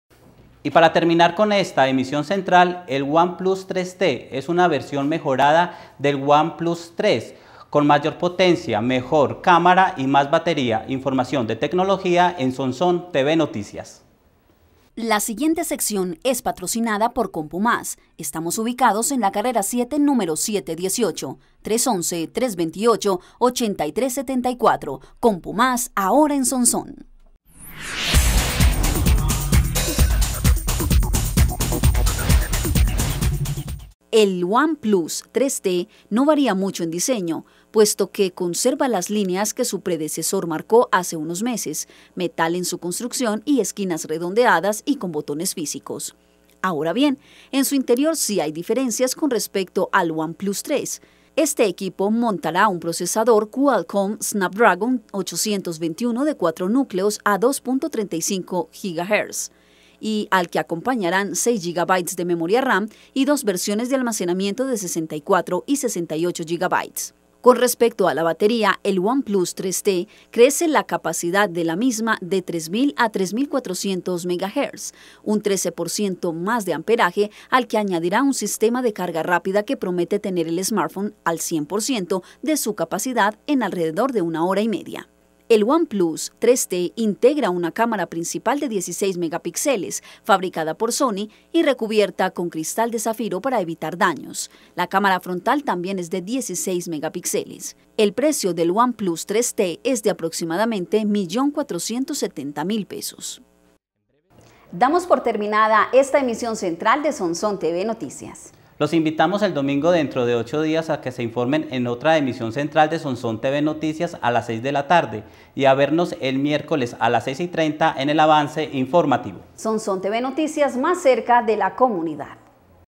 EL ONE PLUS 3T ACABA DE SER PRESENTADO
El One Plus 3T es una versión mejorada del One Plus 3 con más potencia, mejor cámara y más batería.